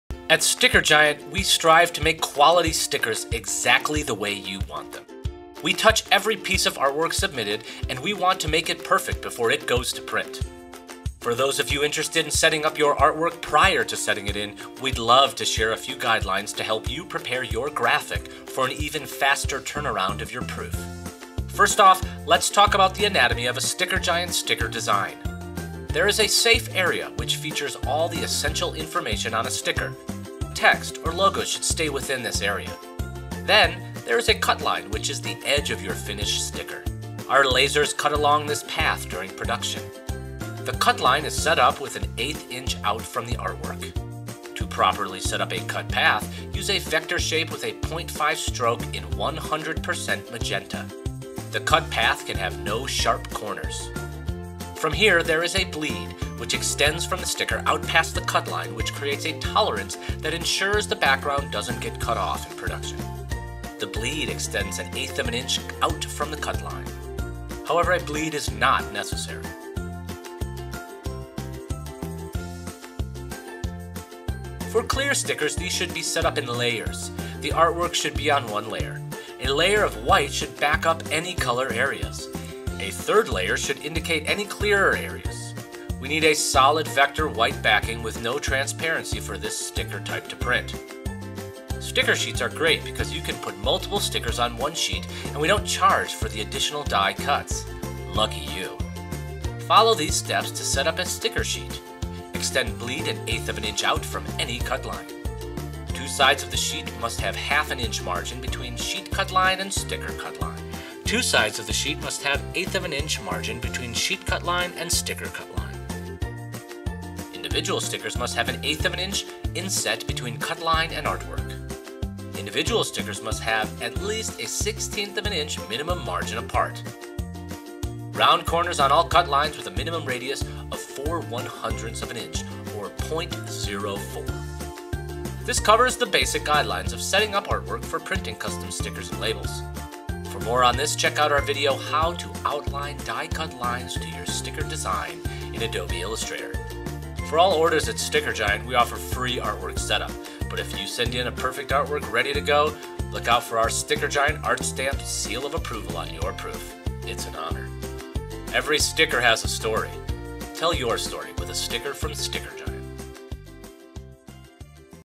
Artwork Guidelines
We offer Free Artwork Setup at StickerGiant, but sometimes it's nice to know how we prepare our art files for print. Here are a few Artwork Guidelines to follow when taking your image and mapping it out to become colorful custom stickers or labels.